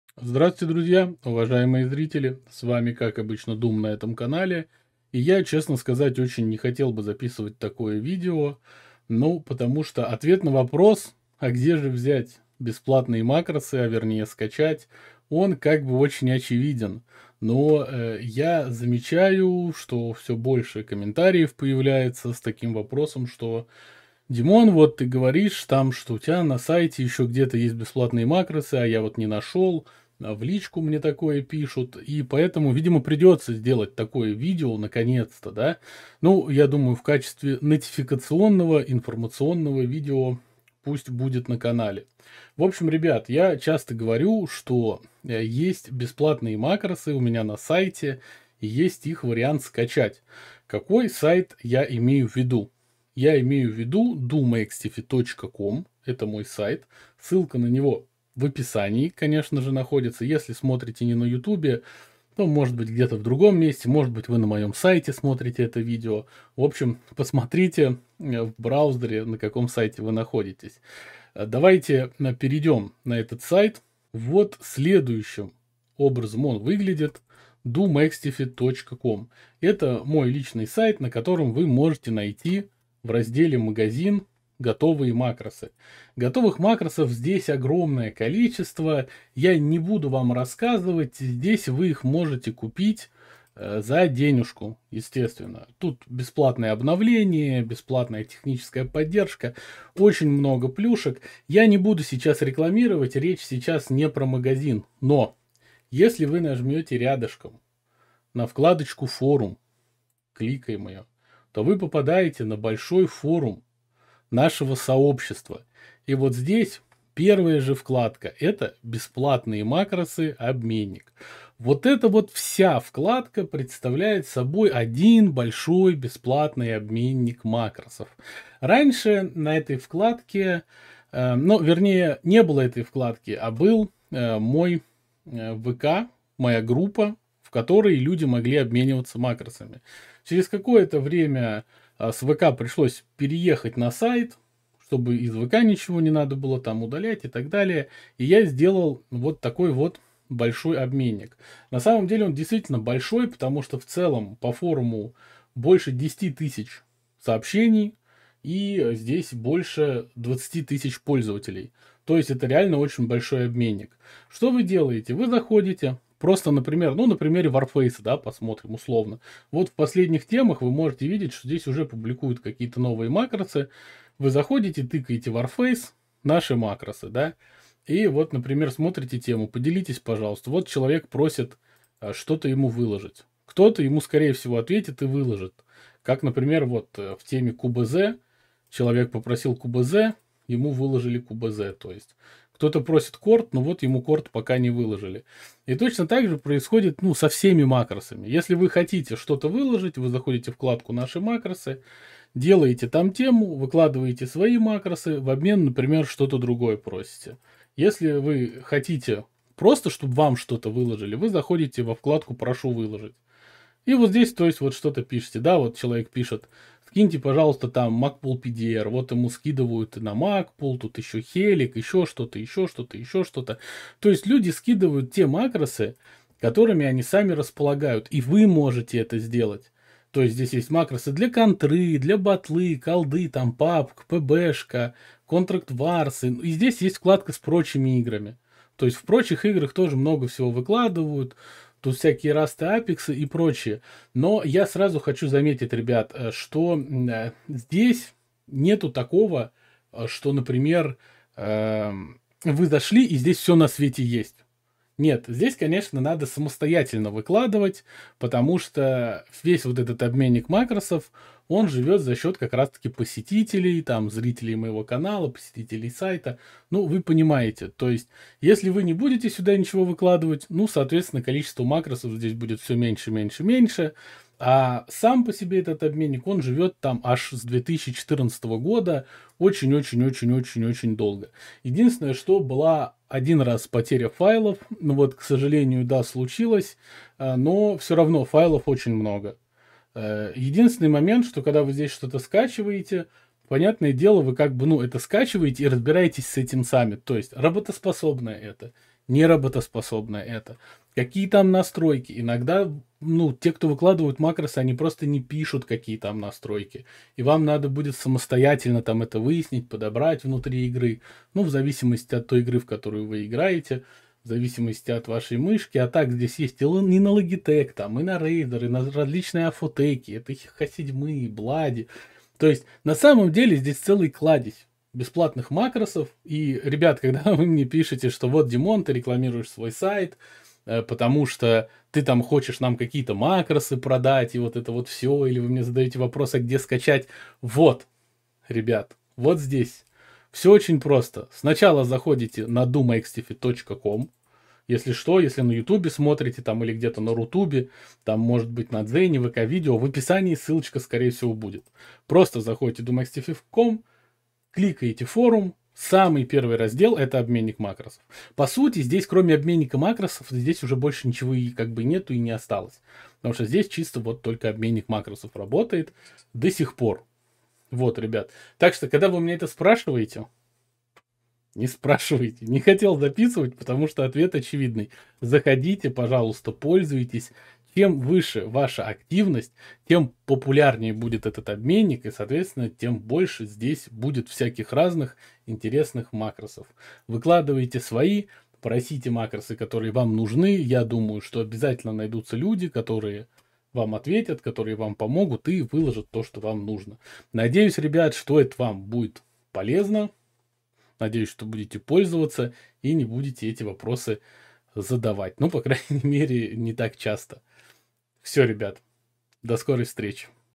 Где скачать макрос бесплатно?
- Установить макрос на мышку Logitech.
- Установить макрос на мышку Razer.
Мои зрители и посетители сайта Дум.хтф задают мне вопрос в комментариях и личных сообщениях, "где скачать макрос бесплатно", я уже устал отвечать отдельно на эти вопросы. Поэтому, решил ответить всем сразу в видео и рассказать, что скачать макросы совершенно бесплатно, можно на моем сайте doomxtf.com в разделе Форум - обменник макросов. Там вы совершенно бесплатно можете скачать макросы, а так же загрузить свои макросы для мышек X7, Bloody, Logitech и Razer, а так же любых других брендов по своему желанию. Надеюсь вопрсов в этом направлении станет меньше, а вы будете получать пользу от обменника макросов.